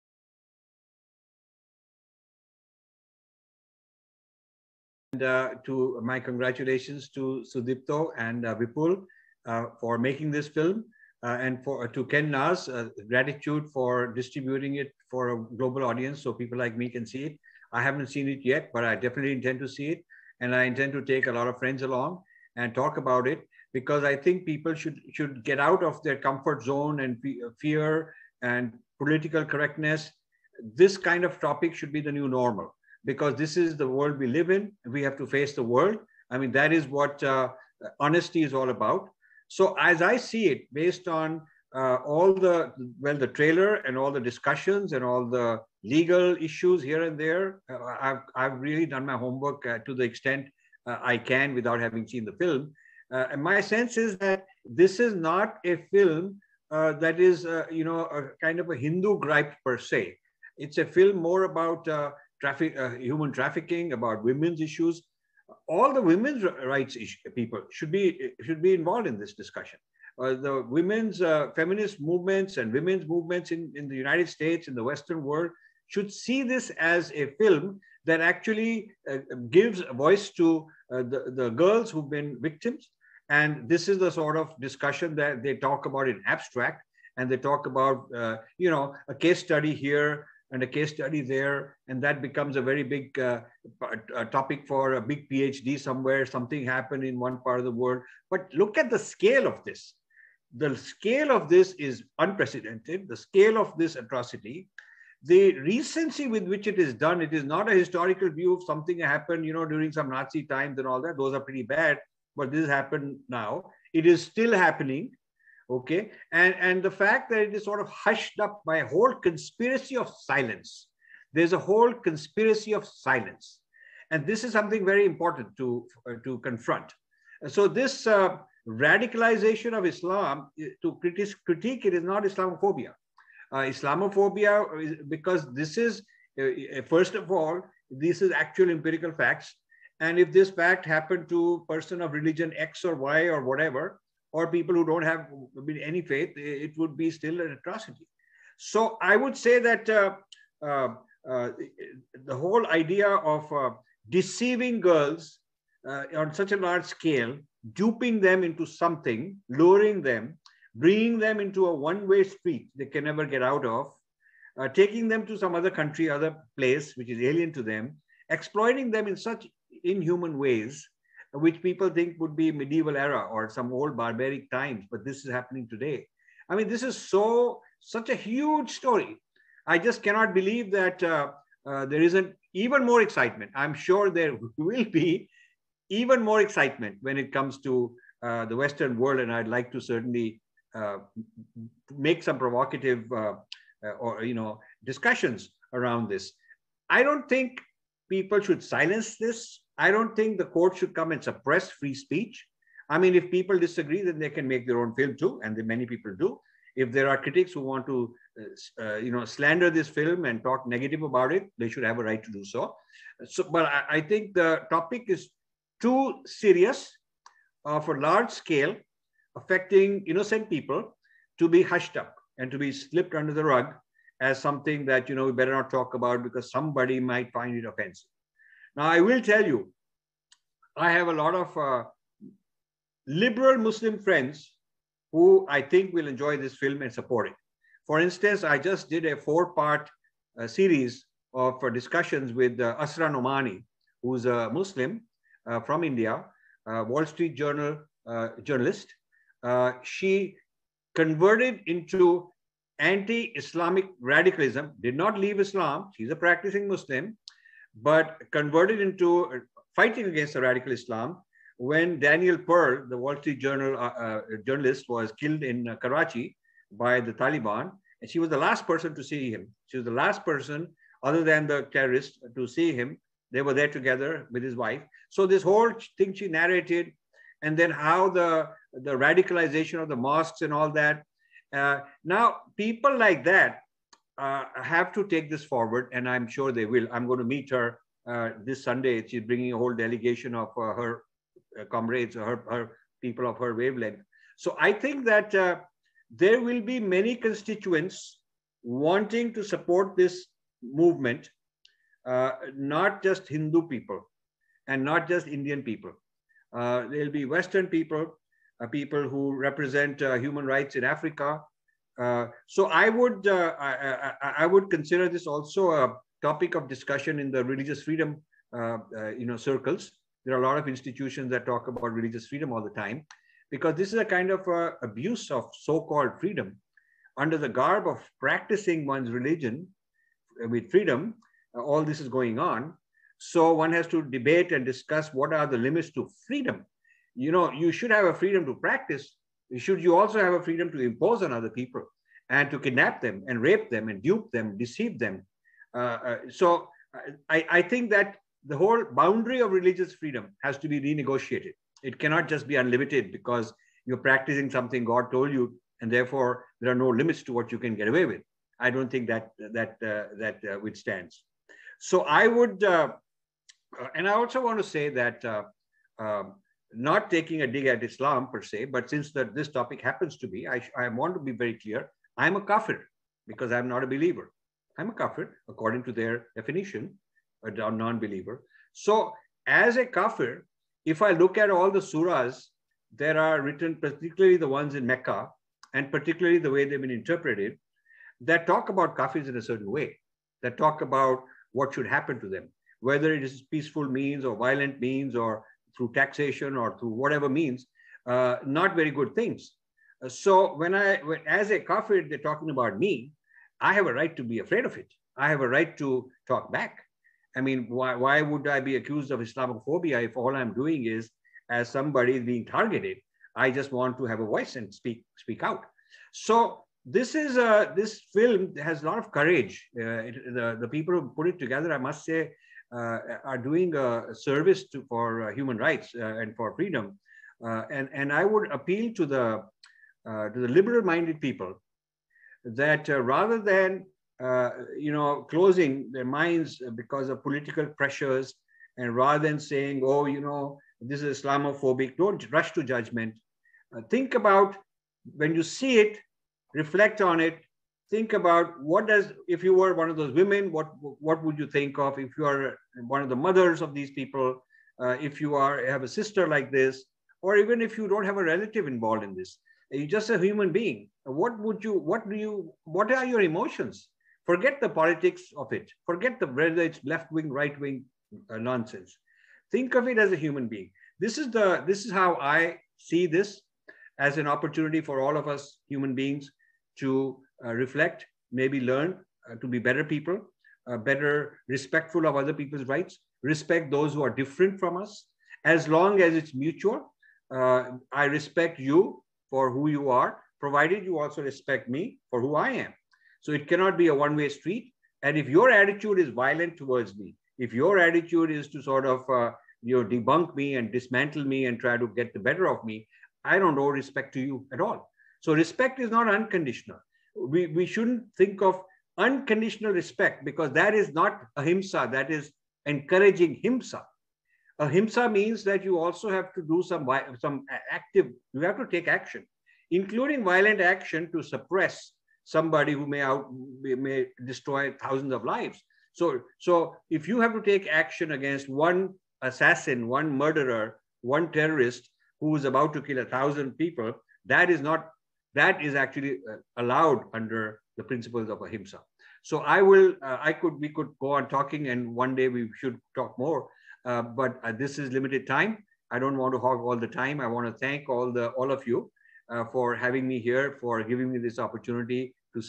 Rajiv Malhotra On thekeralastory
Twitter @RajivMessage: KERALA STORY ENDORSEMENT. I was approached by the publicity agent representing the film's producer, director, lead actress, and international distributor. I was asked to be the main speaker in a video discussion on the movie. The purpose was to get my comments for use in publicity. Below is a clip of my statement. I wish it success in the name of free speech & fearless debate.🙏